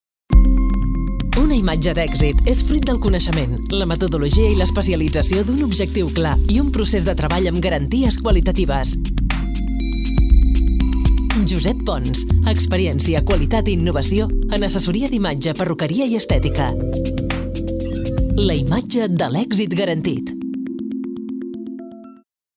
Asesoría y Salons Josep Pons
Josep Pons y parte del equipo español  de natación sincronizada (Andrea Fuentes y Ona Carbonell), nos enseñan las instalaciones de Asesoría y Salones Josep Pons.